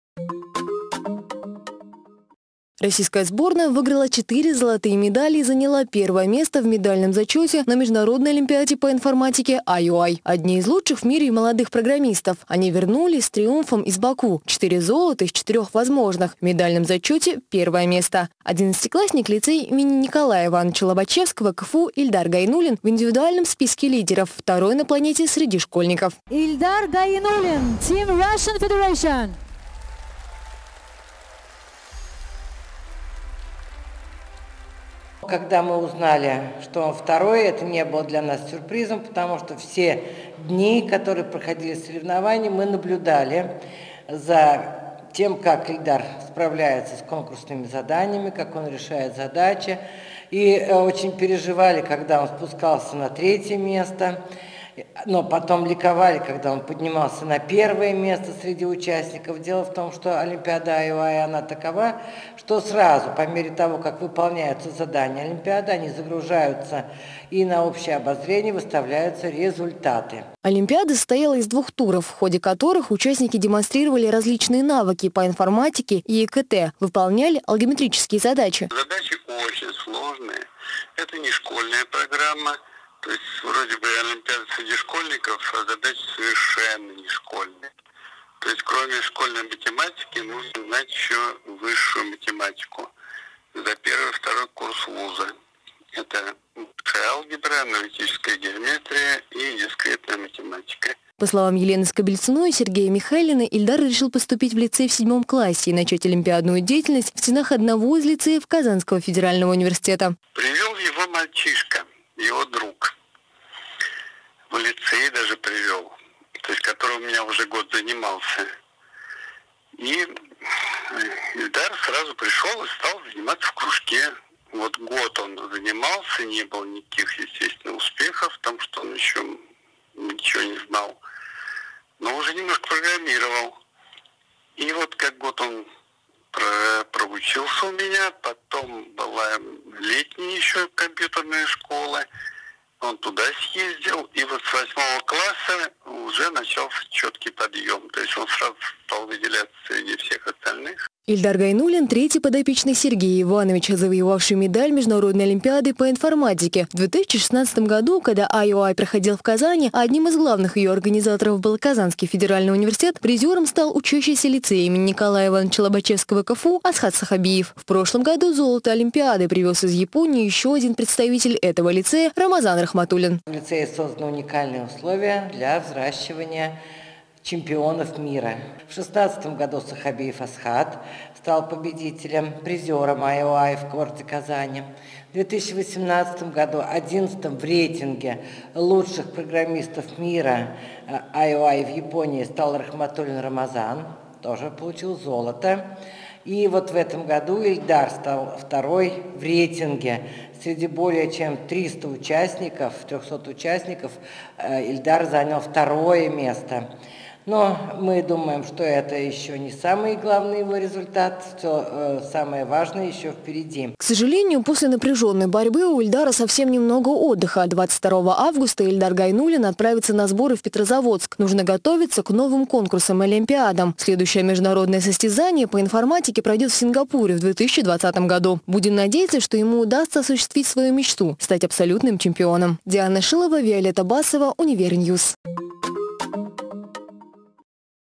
Как становятся чемпионами?
Учащийся 11 класса Лицея имени Н.И.Лобачевского КФУ Ильдар Гайнуллин в составе российской сборной одержал победу на Международной олимпиаде школьников по информатике IOI - 2019, которая проходила с 4 по 11 августа в Баку. Ильдар Гайнуллин внес решающий вклад в победу сборной России, набрав 511,22 балла. О том, каким был путь одного из лучших в мире молодых программистов. Узнаешь в сюжете Дианы Шиловой.